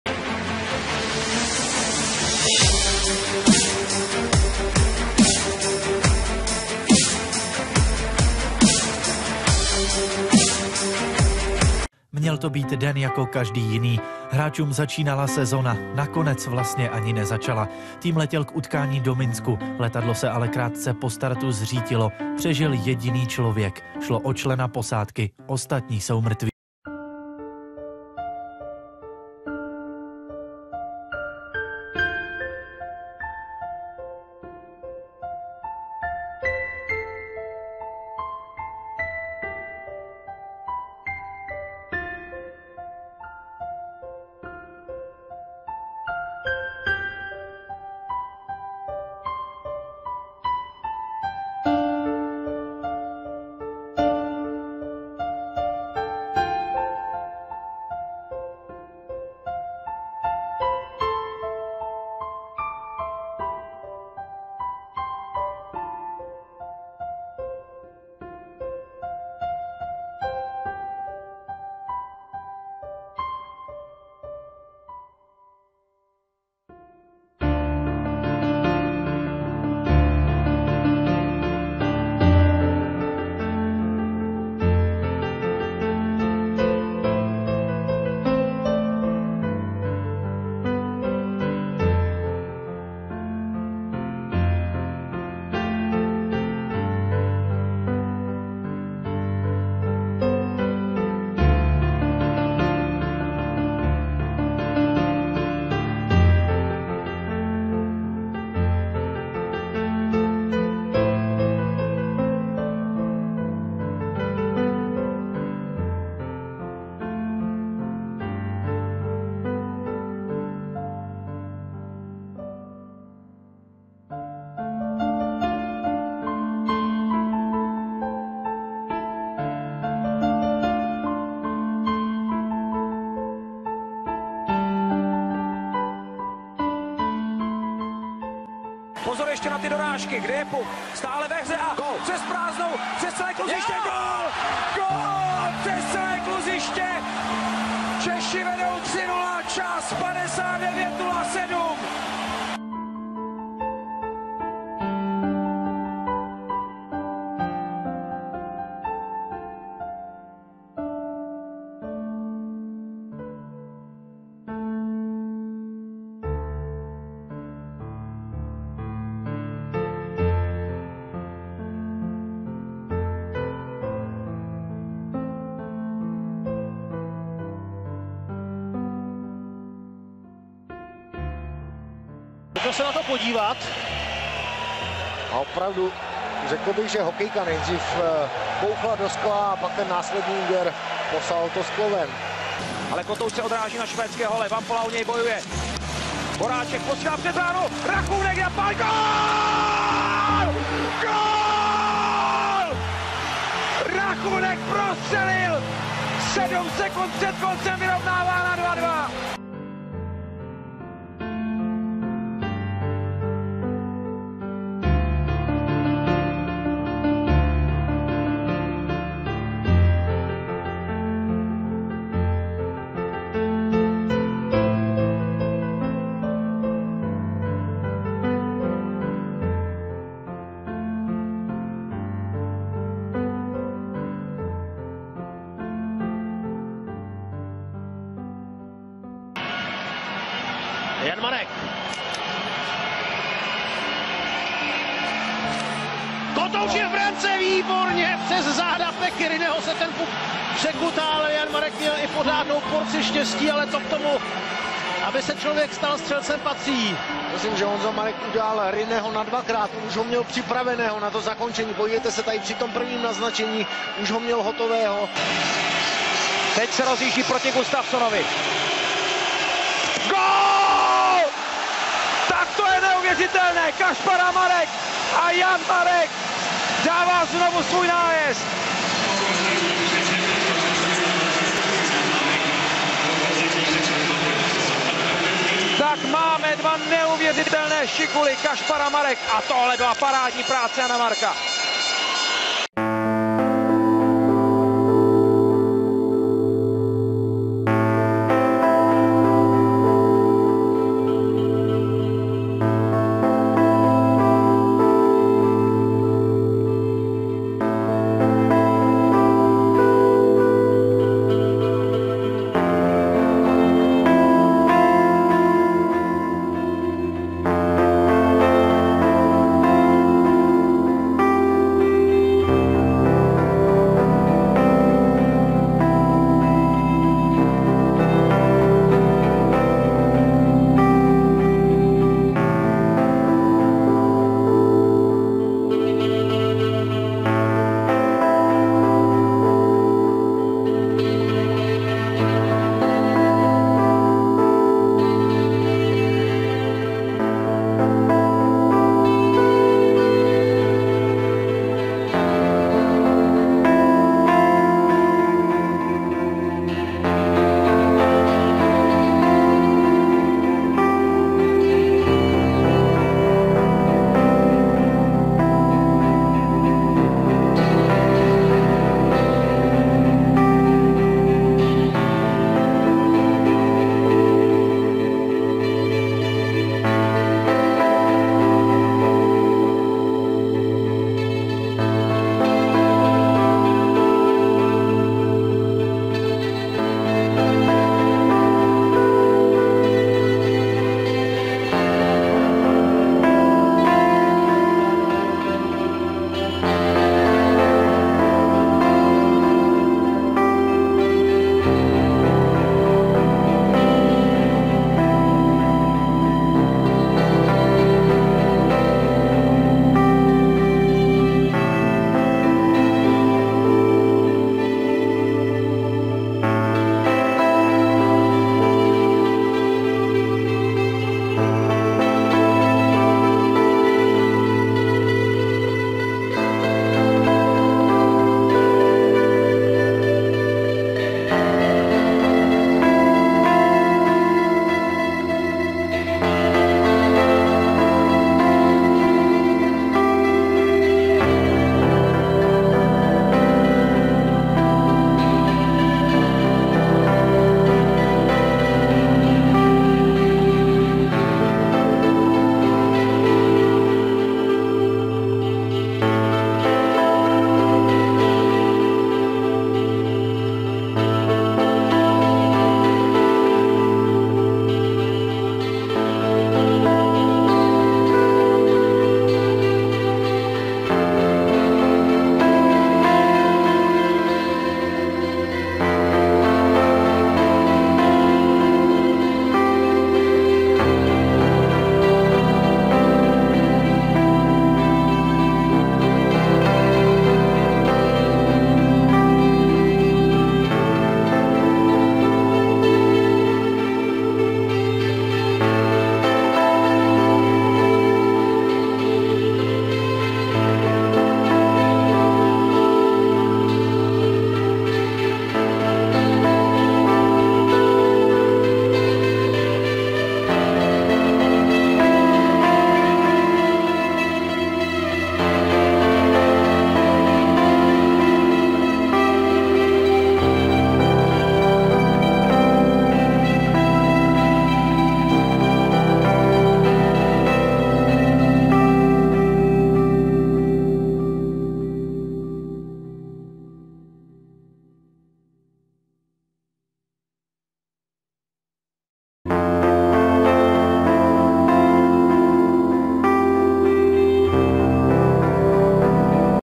Hokejová tragédie 7. 9. 2011 u Jaroslavli † R.I.P. †
Zdravím vás u nového videa, bohužel smutného videa.. :/ 7. září to budou 3 roky od smutné a tragické události, která zasáhla asi nás všechny.. Šlo o pád letadla u města Jaroslavl v Rusku, které vzlétávalo na 1. zápas KHL do Minsku v Bělorusku, s celým týmem Lokomotiv Jaroslavl, za který hráli i 3 čeští hokejisté: Karel Rachůnek, Jan Marek a Josef Vašíček.. Také slovenský reprezentační kapitán Pavol Demitra.. Pád letadla přežil pouze mechanik.. Tímto videem bych chtěl všem příbuzným, známým, přátelům všech osob, které byli na palubě letadla, upřímnou soustrast.. Dál to asi nepotřebuje komentář.. :(